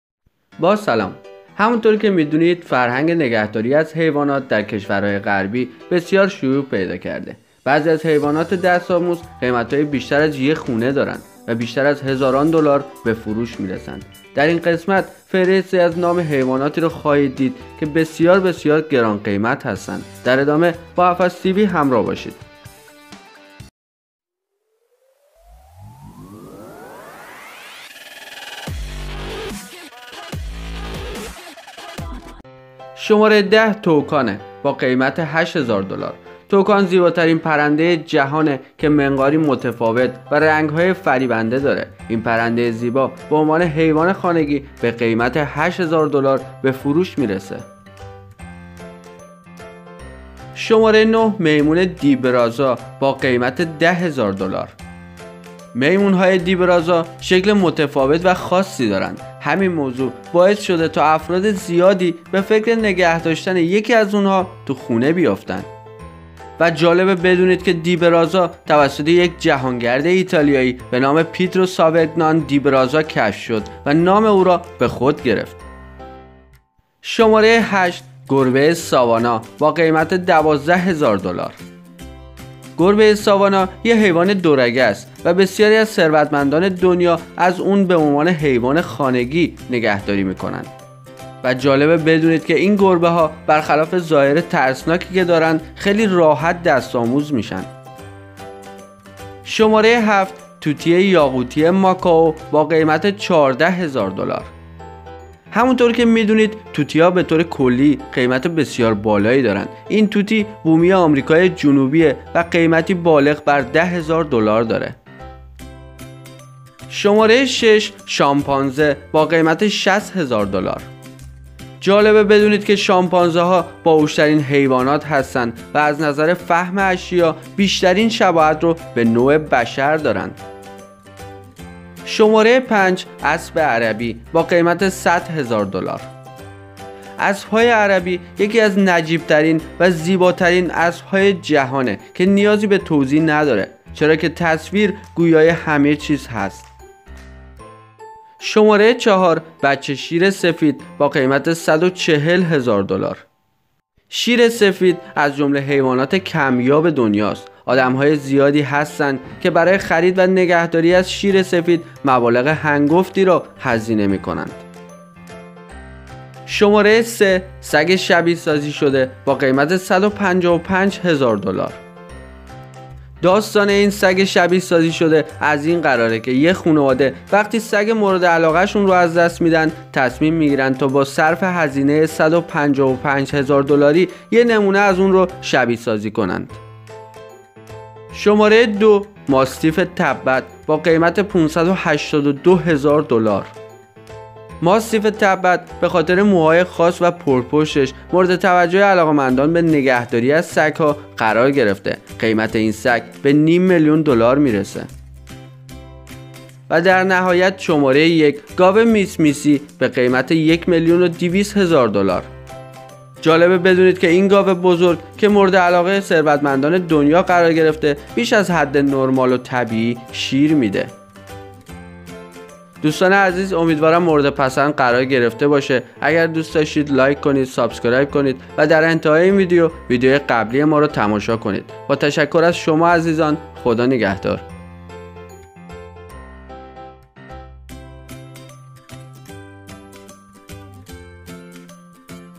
۱۰ تا از گرانترین حیوانات دست آموز
فرهنگ نگهداری از حیوانات در کشورهای غربی شیوع پیدا کرده است. بعضی از حیوانات دست آموز قیمتی بیشتر از قیمت یک خانه دارند و بیش از هزاران دلار به فروش می رسند. در این گزارش  فهرستی از نام حیواناتی را خواهید دید که بسیار گران هستند.